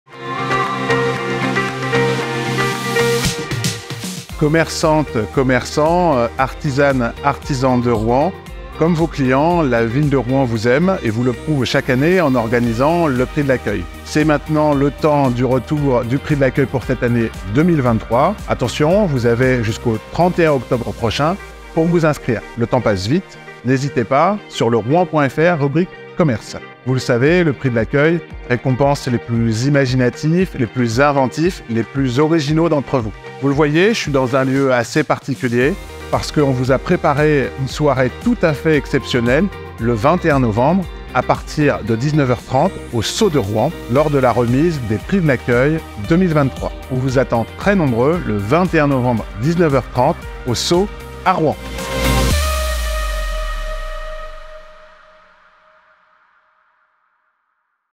Inscription au Prix de l'accueil 2023
Jusqu'au 31 octobre 2023, les commerçants et artisans rouennais qui souhaitent participer à l'édition 2023 du Prix de l'accueil peuvent toujours s'inscrire en ligne.